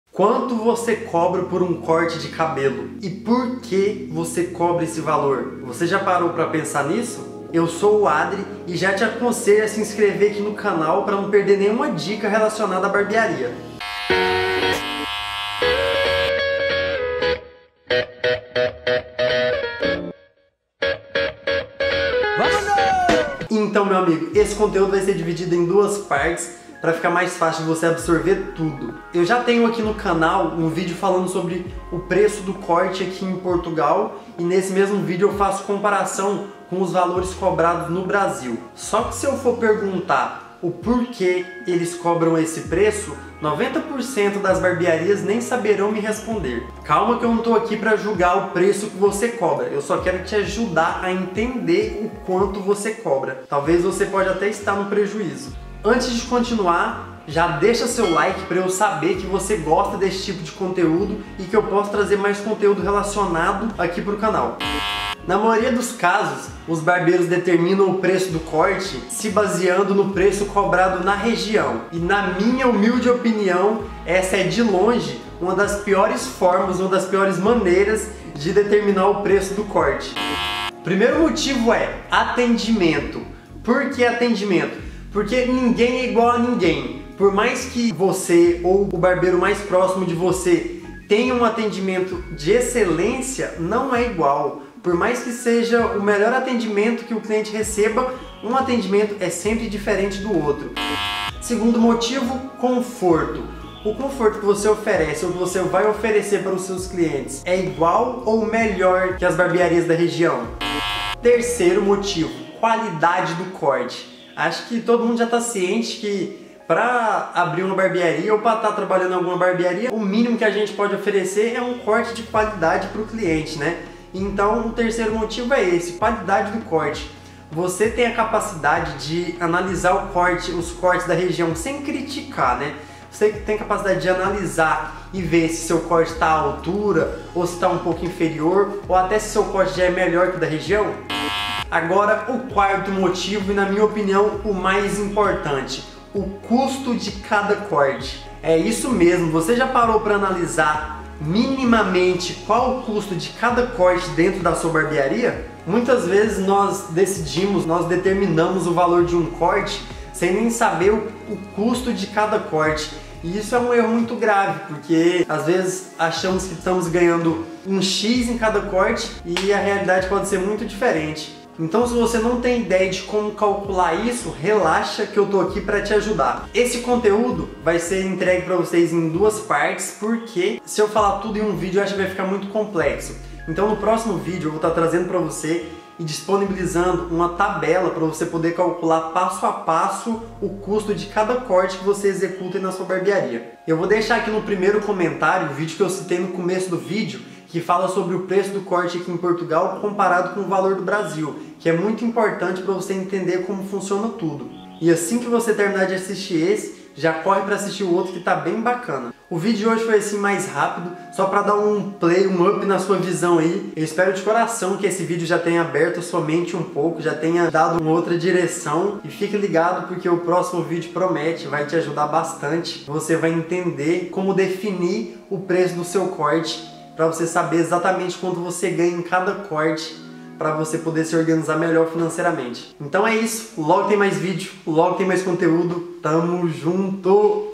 Quanto COBRAR no CORTE de CABELO masculino 💰💈 Dicas de Barbeiro
- Conheça o Barbeiro Digital para conquistar 2x mais clientes em 30 dias

 Quanto cobrar no corte de cabelo masculino? A partir de agora você vai saber como analisar isso!

Se você tem sua Barbearia em Portugal, ou se você tem sua Barbearia no Brasil, essa dica vai te ajudar muito!

Muitos barbeiros não sabem como calcular o preço do corte de cabelo e acabam errando.

Eu sou Barbeiro em Portugal, mas essa dica serve para qualquer barbeiro do mundo!

Tenho certeza que depois desse vídeo você já vai saber Quanto cobrar no corte de cabelo masculino!

Seja um degradê, fade, corte todo na tesoura, corte social, qualquer corte masculino você saberá cobrar o preço certo!

Preço do corte de cabelo, valor do corte de cabelo, quanto cobrar no corte!

💈Material que estou usando atualmente 💈

Tesoura 2: TITAN 3D 6.0 ( 2ª opção no link )